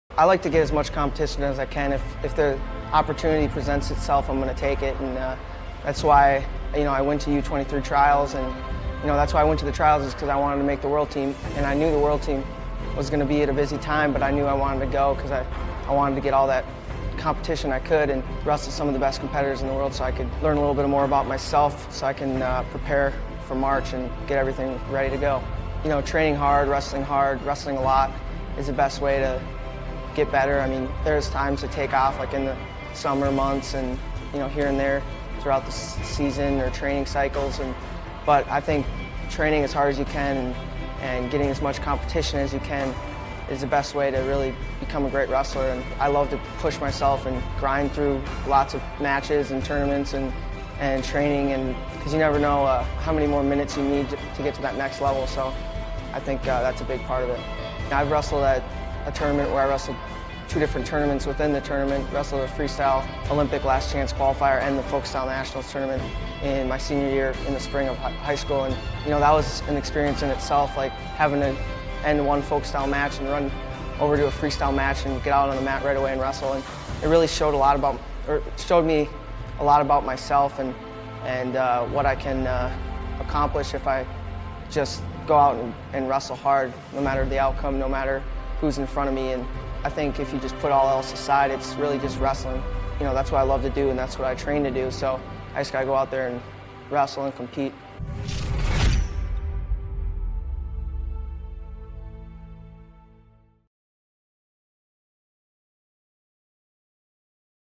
Through the Lens - Mitch McKee
It's been a crazy week and a half for Mitch McKee, but Mitch says you never know how many extra minutes you need to get to that next level. He's the focus of this week's Through the Lens.